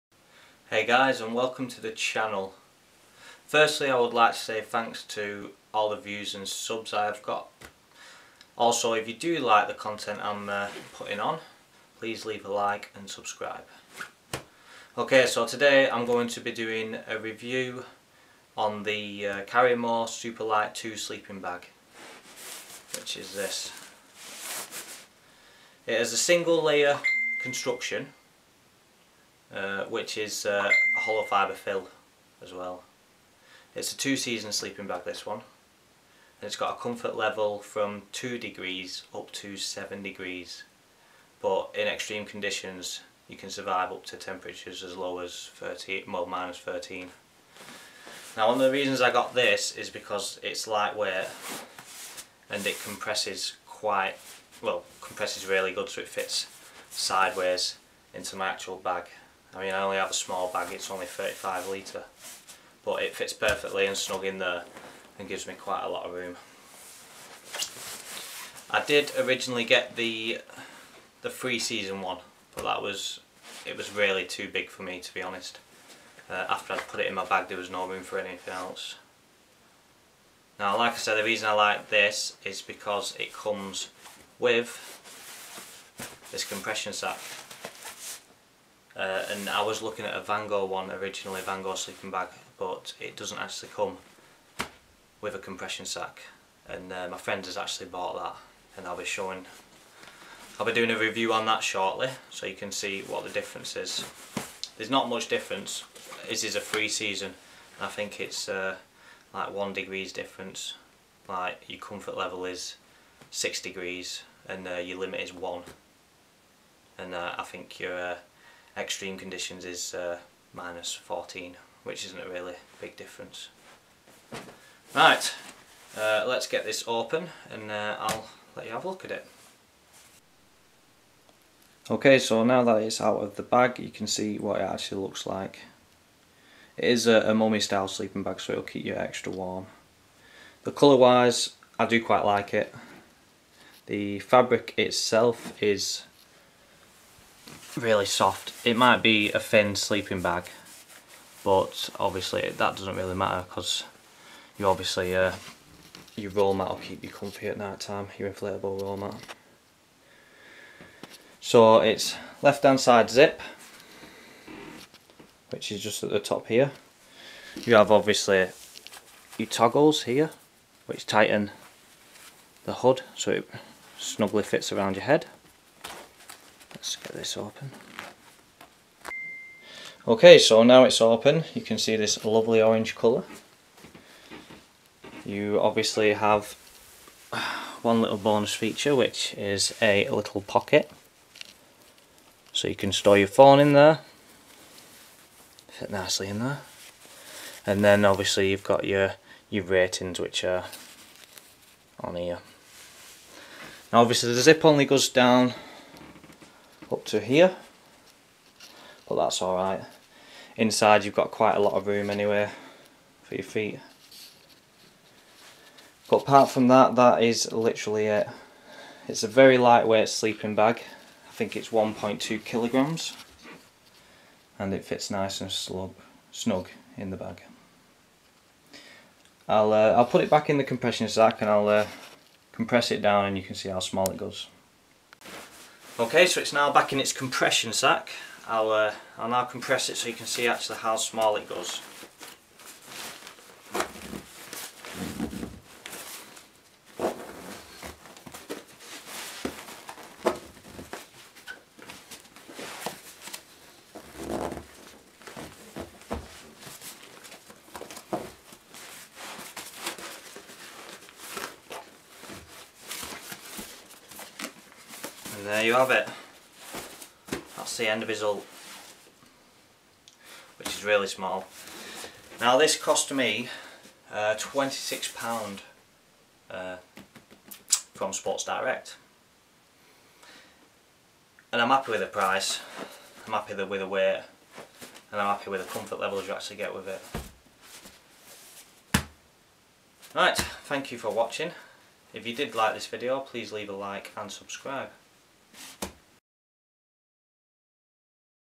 Karrimor Superlight 2 Sleeping bag (First Impressions)
This is just a short video on the krrimor superlight 2 sleeping bag

Links:
karrimor superlight 2